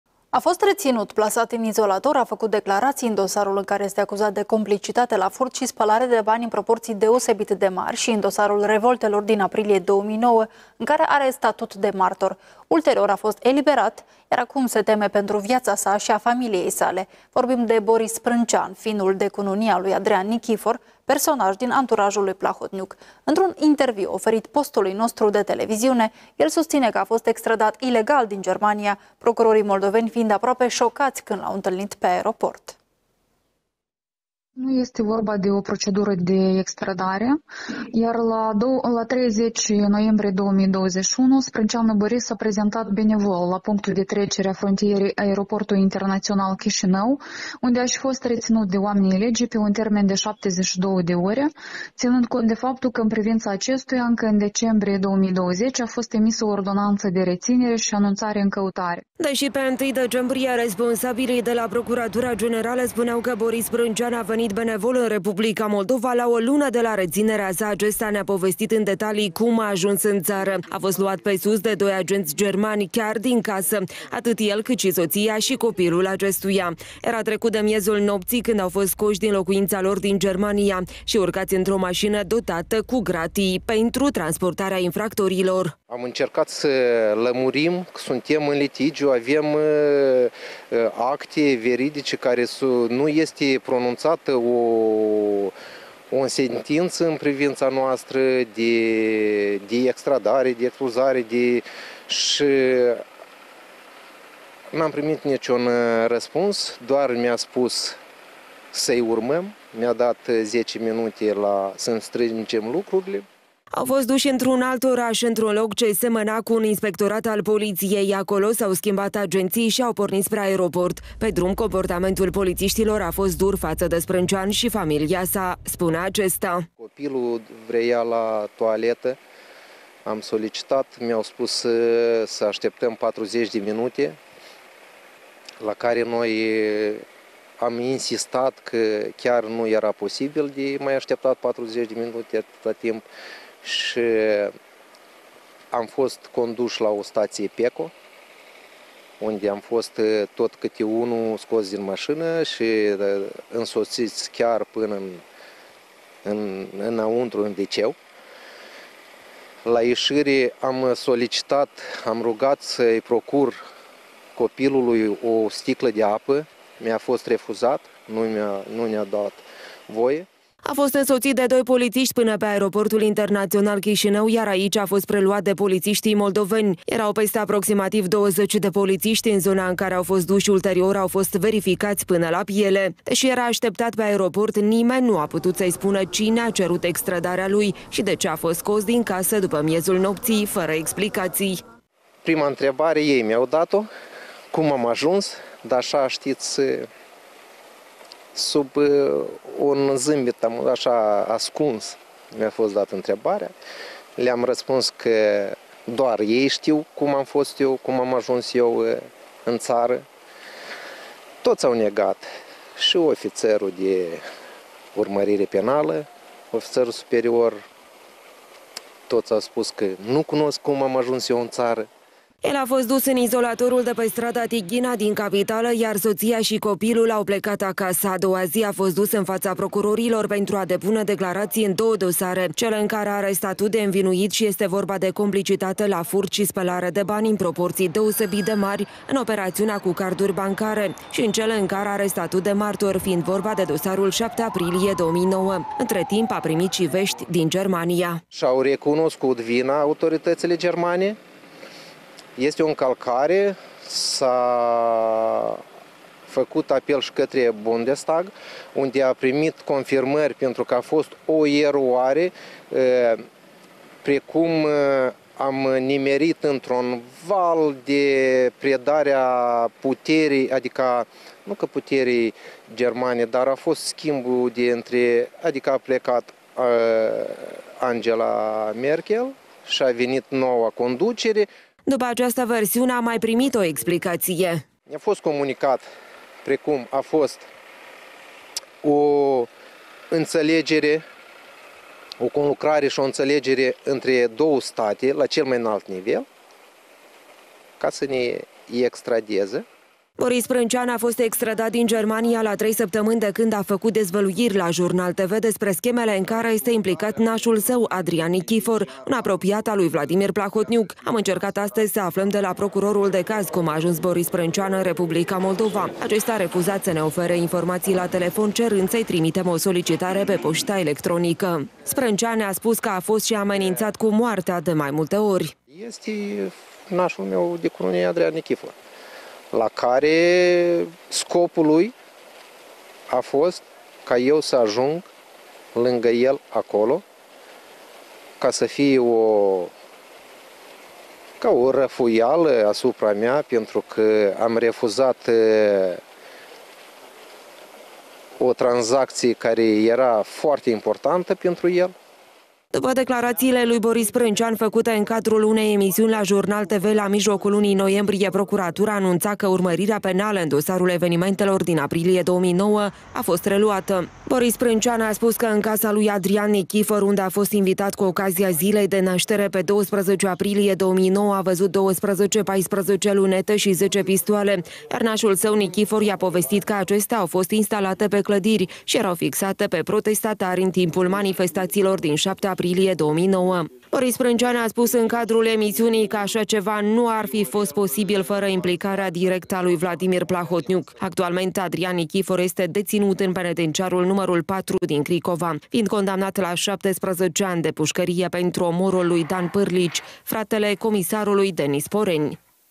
Boris Sprâncean, finul de cununie al lui Adrian Nichifor, susține că a fost extrădat ilegal
A fost reținut, plasat în izolator, a făcut declarații în dosarul în care este acuzat de complicitate la furt și spălare de bani în proporții deosebit de mari și în dosarul revoltelor din aprilie 2009, în care are statut de martor. Ulterior a fost eliberat, iar acum se teme pentru viața sa și a familiei sale. Vorbim de Boris Sprâncean, finul de cununie al lui Adrian Nichifor - personaj din anturajul lui Plahotniuc. Într-un interviu oferit postului nostru de televiziune, el susține că a fost extrădat ilegal din Germania, procurorii moldoveni fiind aproape șocați când l-au întâlnit pe aeroport.